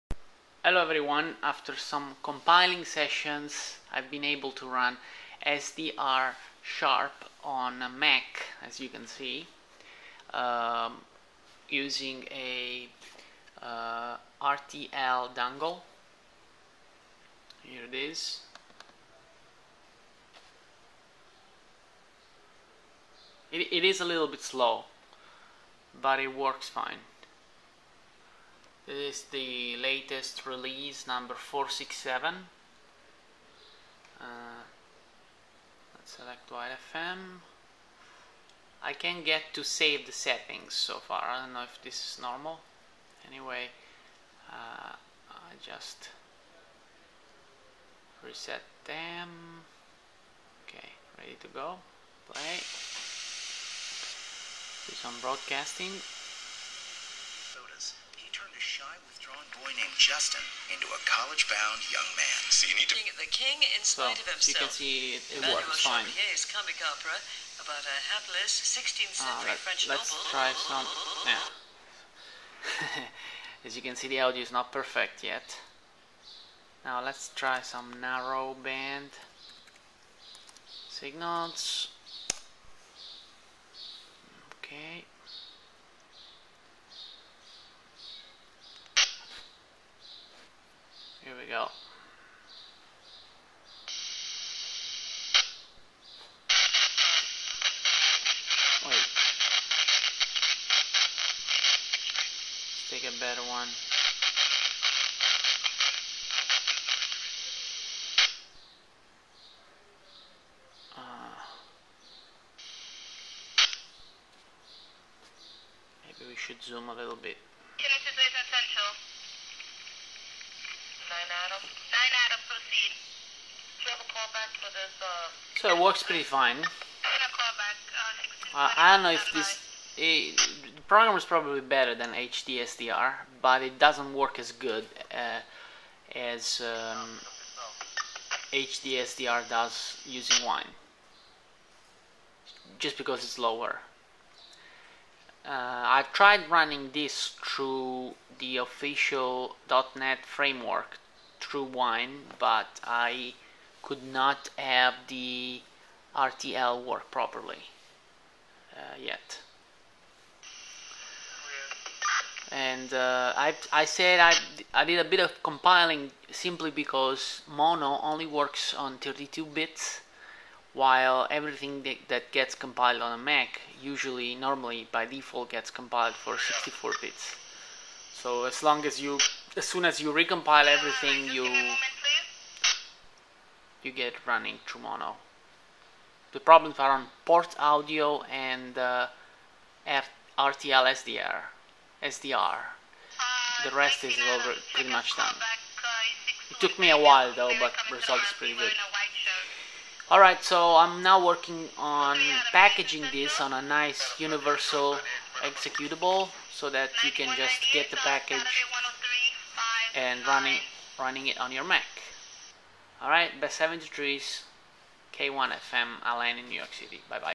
SDR# and RTL2832U running on OSX 10.7.4 via Mono
Mono JIT compiler version 2.10.9
SDR# version Rev 467

just compile everything that is causing errors (portaudio and librtlsdr) as i386 (or universal) and it will work